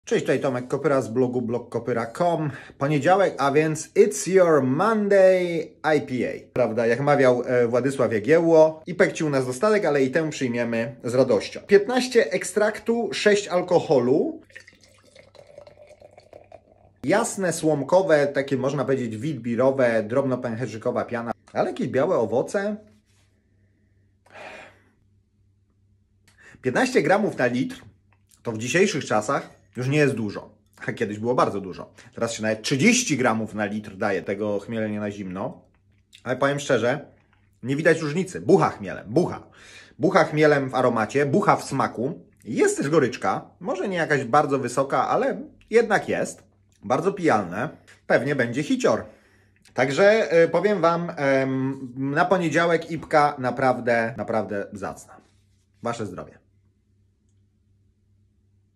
Psst... It's Your Monday IPA - Browar PINTA
Tomasz Kopyra w shortdegu sprawdza Psst... It's Your Monday IPA z Browaru PINTA
DDH Hazy IPA
Ekstrakt: 15% wag.
Alkohol: 6% obj.
Hoprate: 15g/l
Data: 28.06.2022
Cena: 8,49 zł (Lidl)